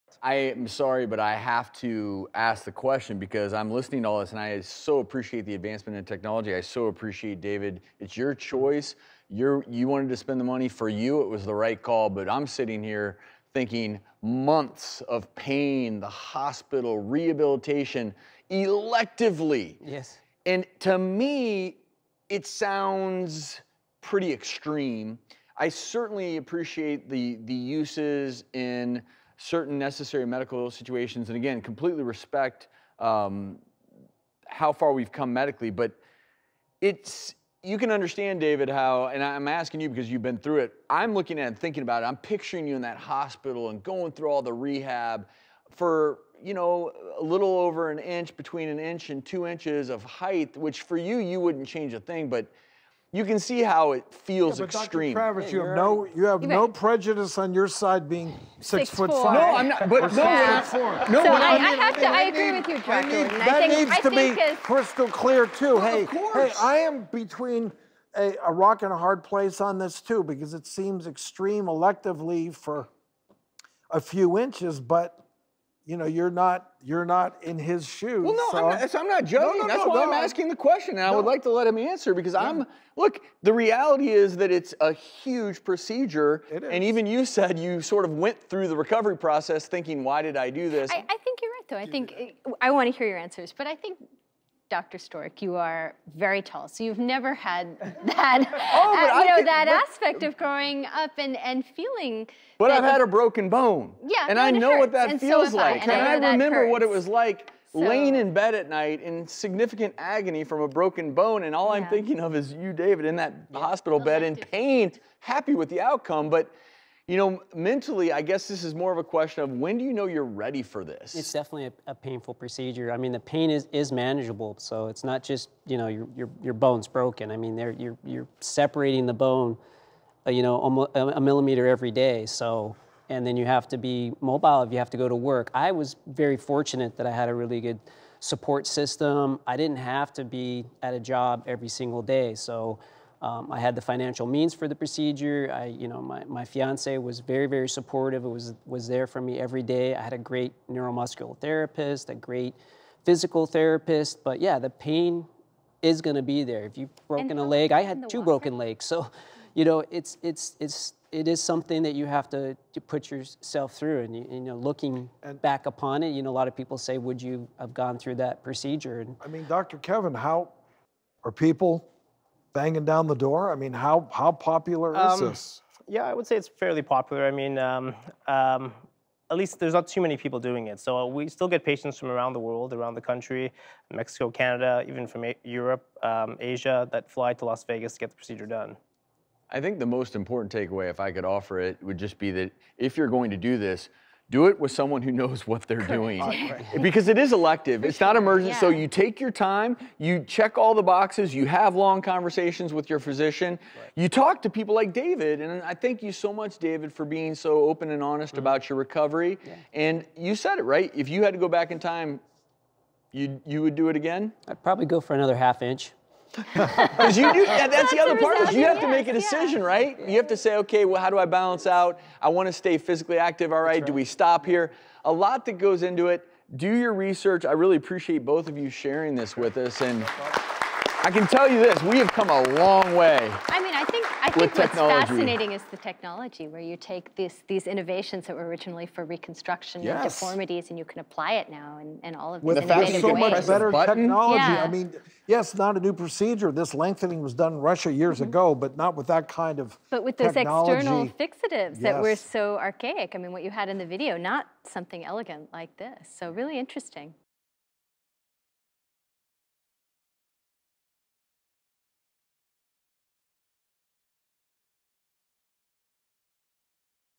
Man Describes Painful Limb-Lengthening Procedure to Get Taller
David underwent a cosmetic procedure to gain some extra height going from 5’7 to 5’9, which he was very happy with. David admits that the recovery was long and hard, but he’s very happy with the results and shares that he would do it again. Orthopedic surgeon Dr. Kevin Debiparshad shares that it’s a popular procedure in his practice, but ER physician Dr. Travis Stork shares that it definitely sounds extreme.